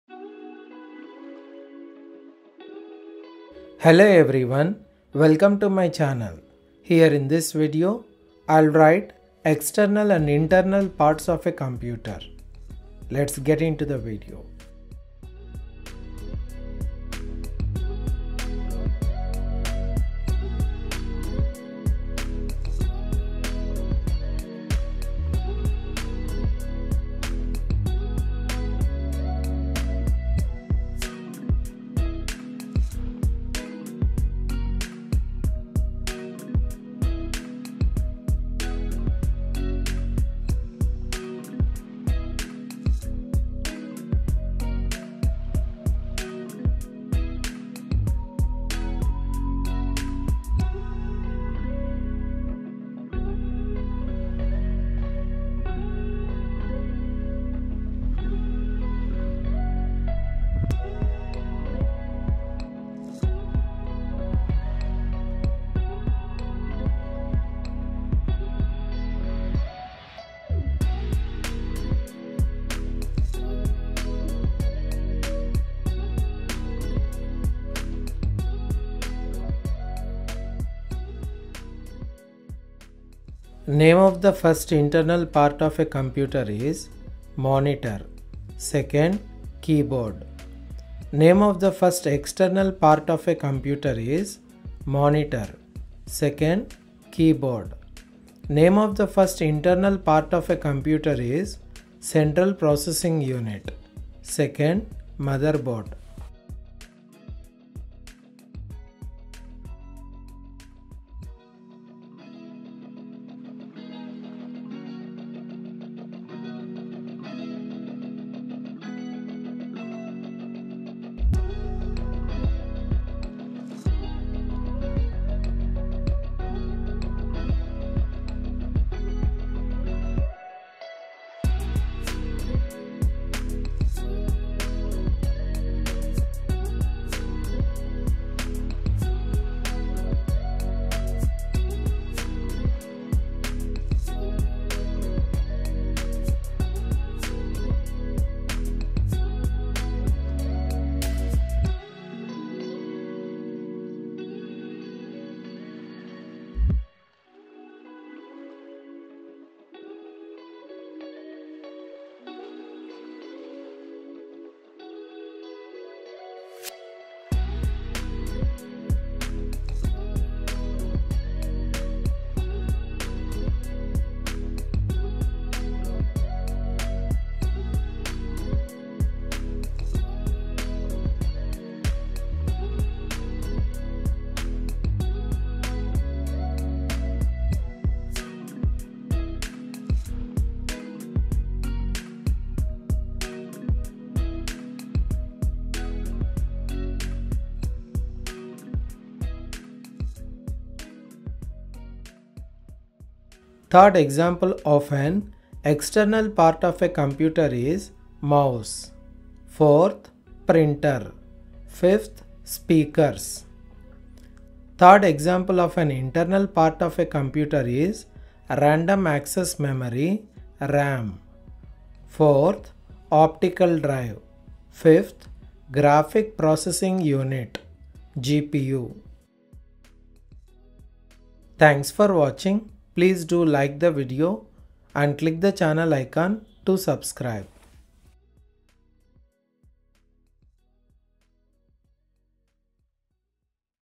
External and Internal Parts of the Computer | 5 External and Internal Parts of the Computer
Looking for a video on External and Internal Parts of a computer? Check out this video with 5 External and Internal parts of a computer in English, which explains the topic easily.

external and internal parts of a computer
5 external and internal parts of a computer